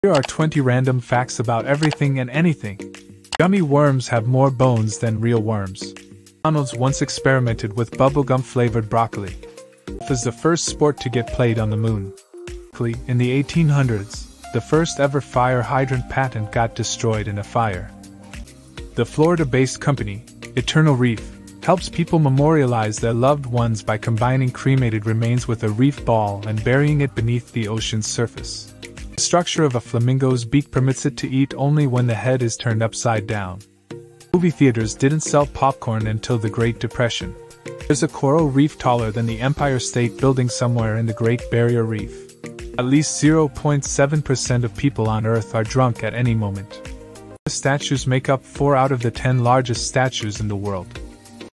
23# - Facts You Never Knew About the World Around Us
23#  - Facts You Never Knew About the World Around Us

Here are random facts about everything and anything. Gummy worms have more bones than real worms. McDonald's once experimented with bubblegum flavored broccoli. Golf is the first sport to get played on the moon. Ironically, in the 1800s, the first ever fire hydrant patent got destroyed in a fire. The Florida-based company, Eternal Reef, helps people memorialize their loved ones by combining cremated remains with a reef ball and burying it beneath the ocean's surface. The structure of a flamingo's beak permits it to eat only when the head is turned upside down. Movie theaters didn't sell popcorn until the Great Depression. There's a coral reef taller than the Empire State Building somewhere in the Great Barrier Reef. At least 0.7% of people on Earth are drunk at any moment. Buddha statues make up four out of the ten largest statues in the world.